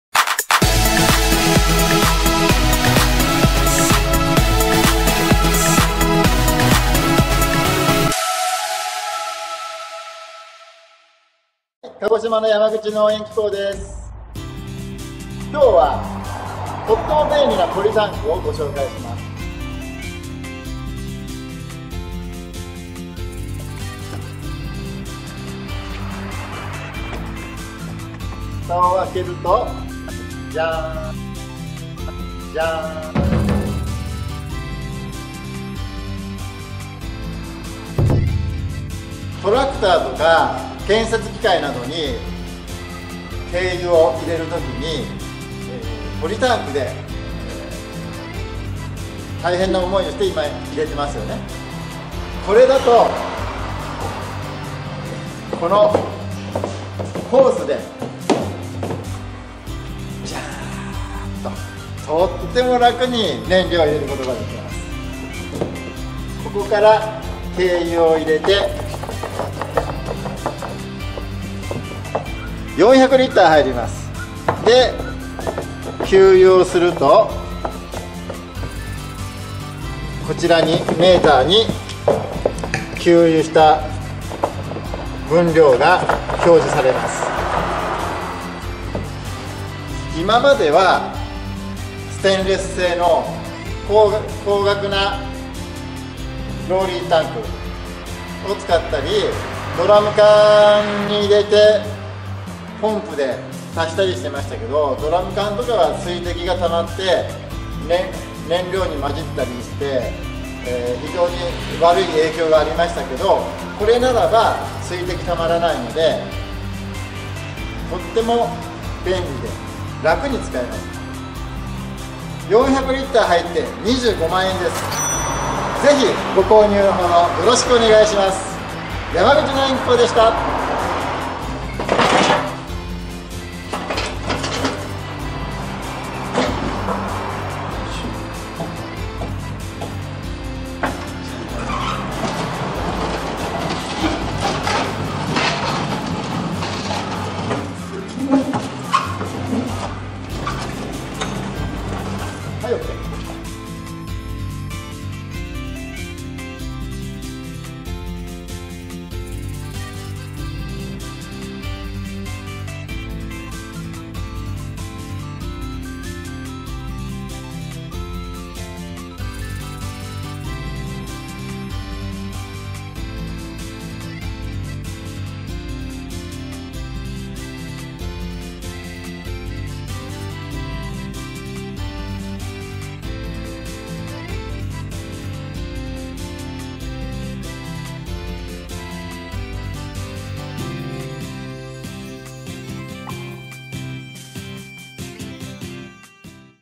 軽油専用タンク ゲスパサ ディーゼルポーリータンク《在庫分完売》予約受付中！！
《在庫分完売》次の入荷予定は５月ですが、注文受け付けています。
建設機械やトラクター等の給油に便利な軽油専用のタンクをご紹介します。
保存・運搬・給油、1台3役！
※動画内でDC152Vモーターとなってますが12ｖの間違いです。すみません。
※法人でお届け先にフォークリフトがある場合、メーカー直送で送料無料です。
フォークリフトがなく個人の場合、最寄りの営業所止めとなります。詳しくはご相談ください。

容量：410L　横幅(mm)：1150
奥行(mm)：760 　高さ(mm)：820 重量(kg)：61